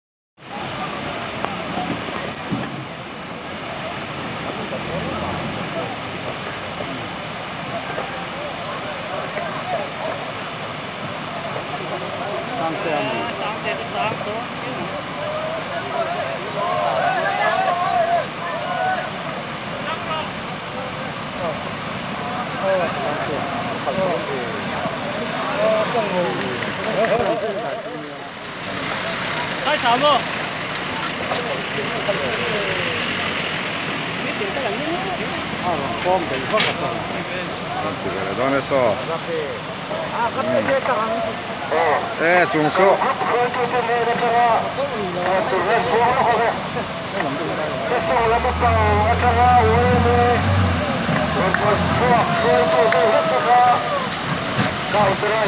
Chalfilh Group YMA hnatlang pui Dt.14.5.2018 Thawhṭanni
Vawiin Dt.14.5.2018 hian Chalfilh Group YMA te chuan Tuirini lei chim avanga Kal kai na tur diversion siamin hnatlangpui an nei a member 1300 chuang branch 17 aṭangin an hnatlang.